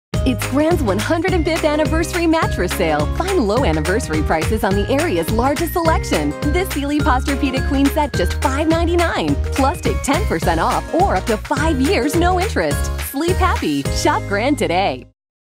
Anniversary Mattress Sale
Find low anniversary prices on the area's largest selection. This Sealy Posturepedic queen set now $599!